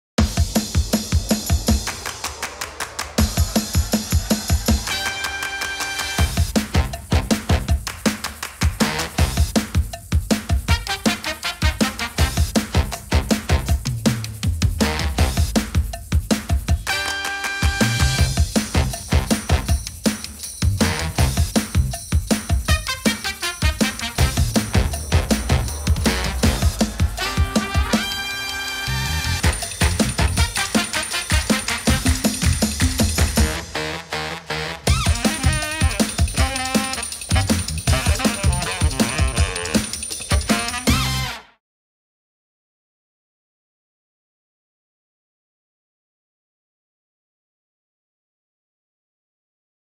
Cordon bleu di polenta - Ricetta in 1 minuto
I cordon bleu di polenta sono una ricetta veramente sfiziosa, perfetta da servire come secondo piatto o, in versione mini, per un aperitivo tra amici! ★ INGREDIENTI, DOSI E PROCEDIMENTO